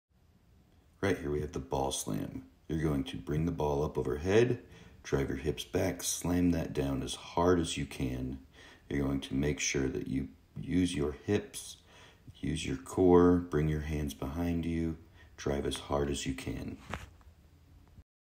Ball slam
With the ball slam start with the ball above your head slam as hard as you can to the ground driving with your hips and core. Bring your hands behind you pick the ball up and repeat. ￼